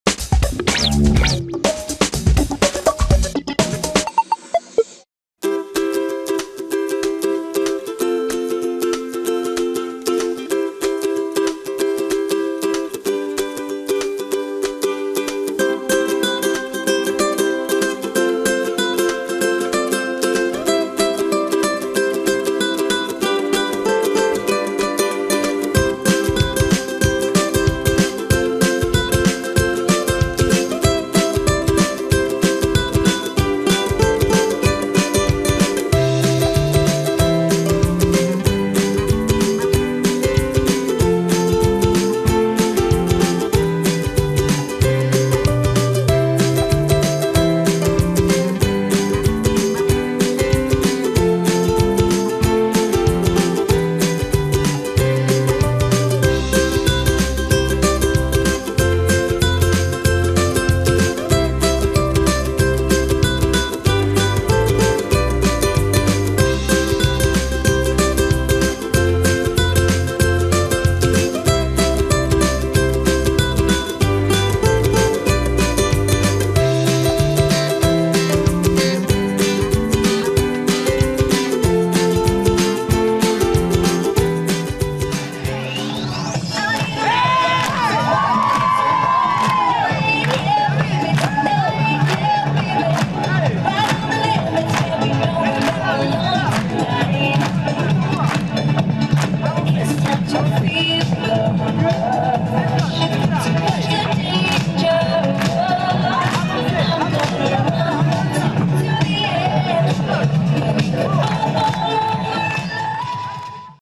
Animations voie sur berge
Retour en images sur l'animation de la voie sur berge le 11 mars 2012.